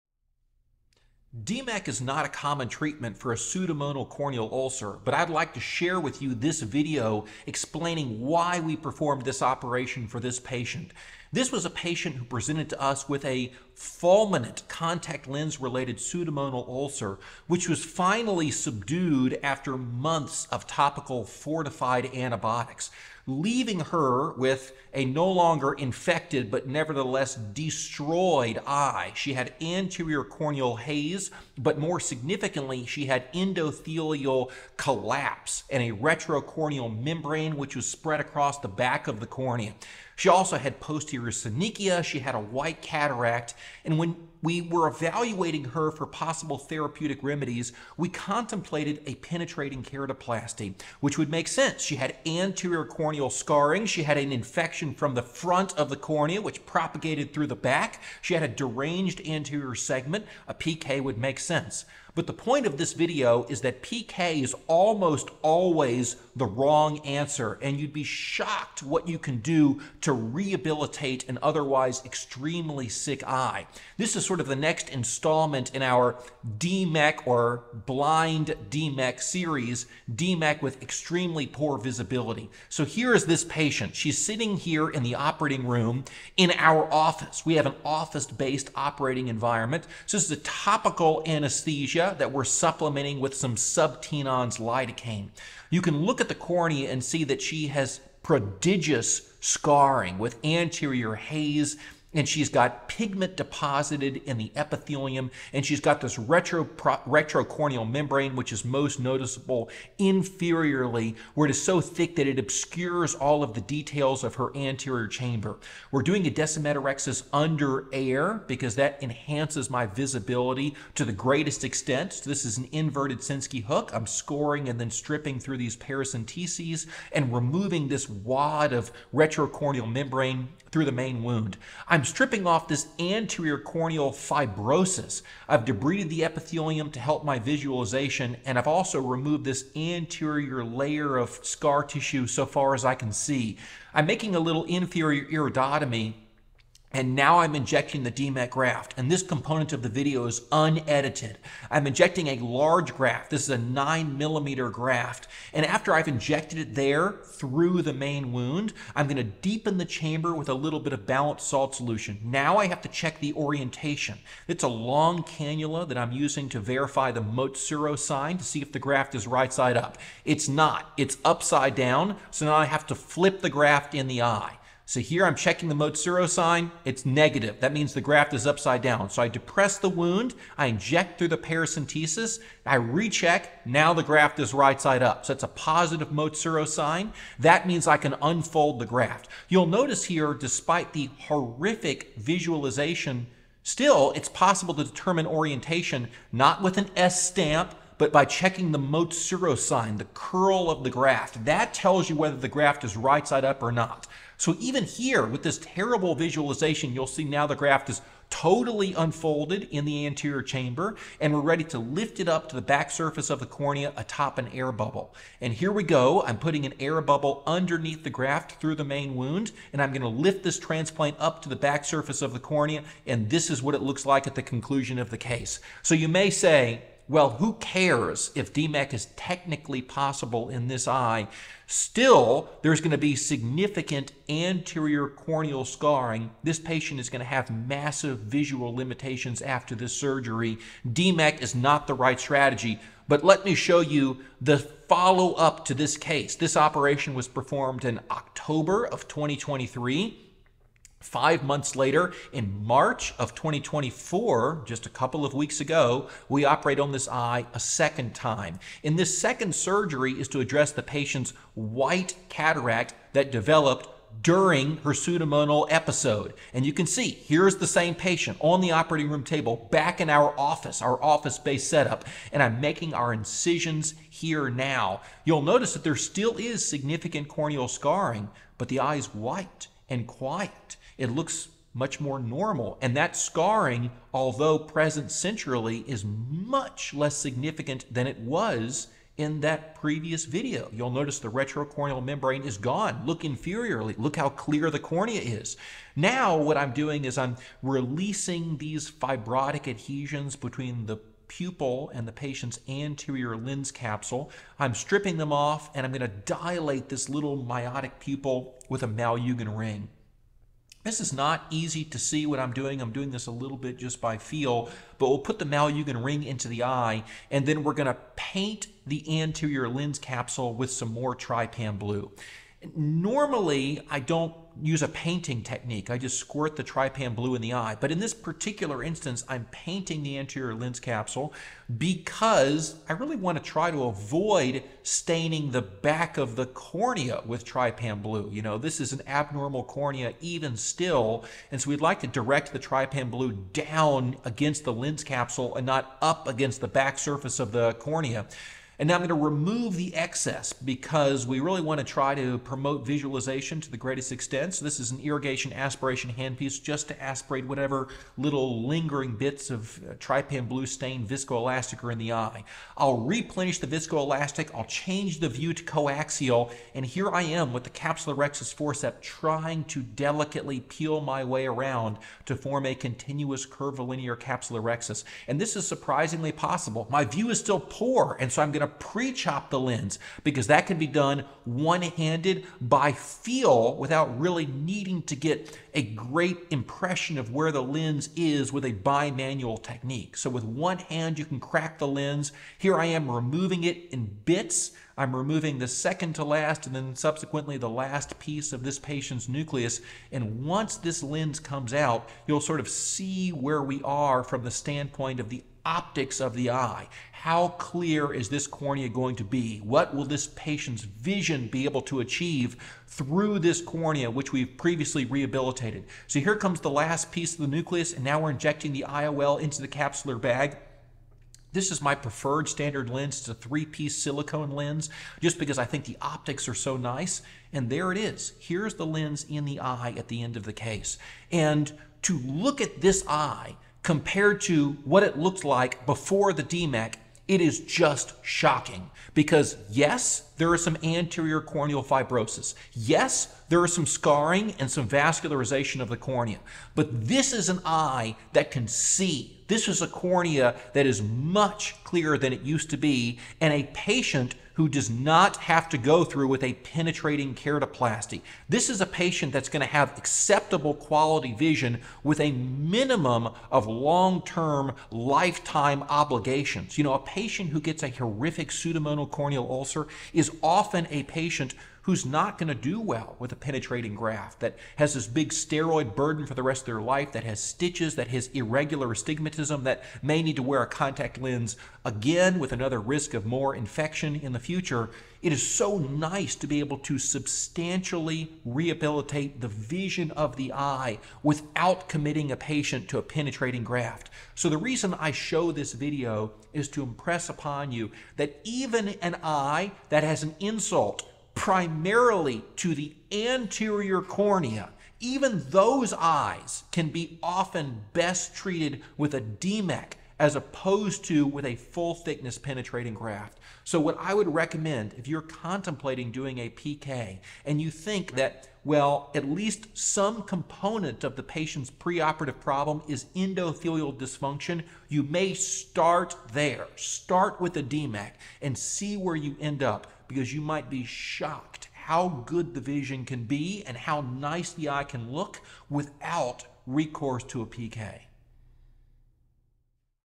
DMEK for Pseudomonal Corneal Ulcer
Pseudomonas is a horrific cause of corneal infection that can cause obliterative damage to the cornea and to the anterior chamber. Here, we show how much corneal haze and scarring can be reversed by DMEK surgery alone, without resorting to a penetrating keratoplasty